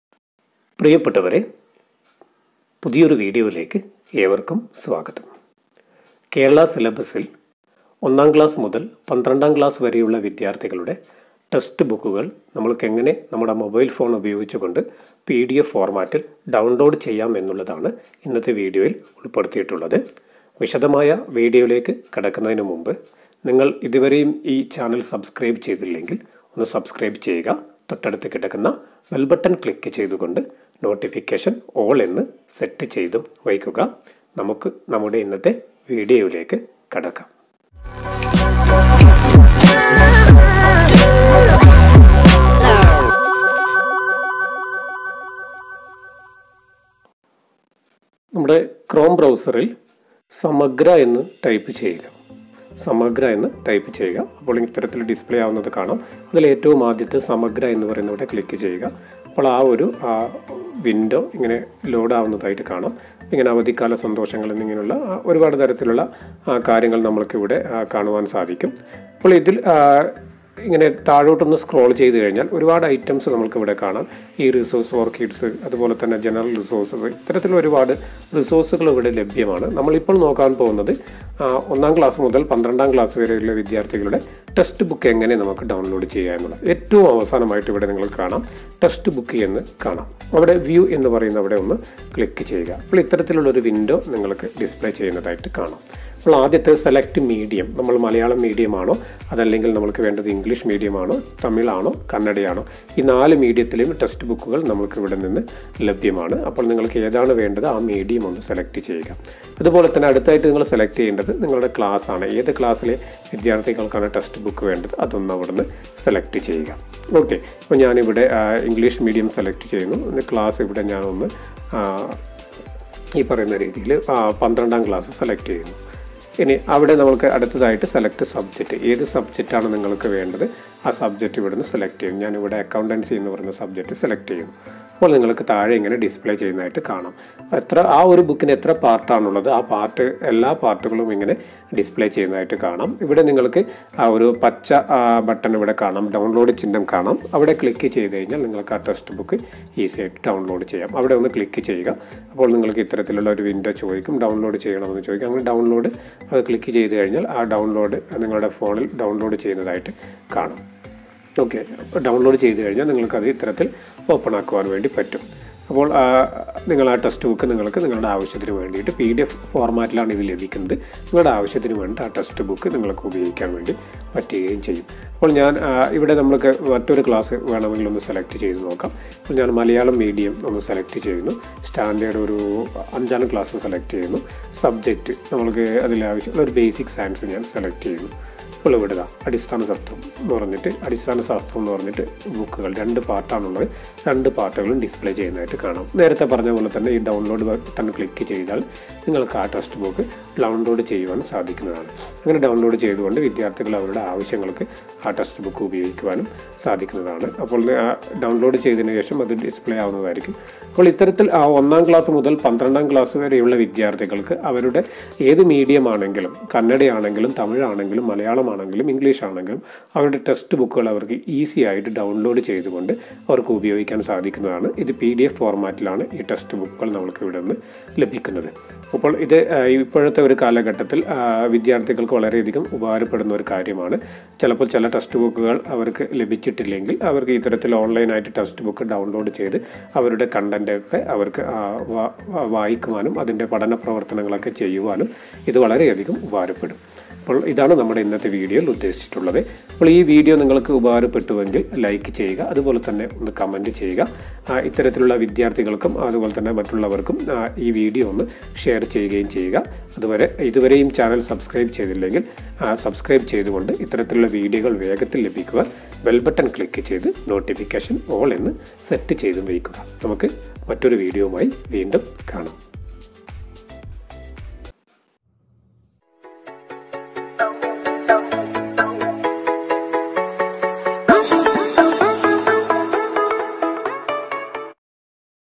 ഒന്ന് മുതൽ +2 വരെ കേരള സിലബസ് text books മൊബൈലിൽ ഡൗൺലോഡ് ചെയ്യാം _ How to dowload text books kerala
How to download Kerala syllabus text books via mobile in pdf format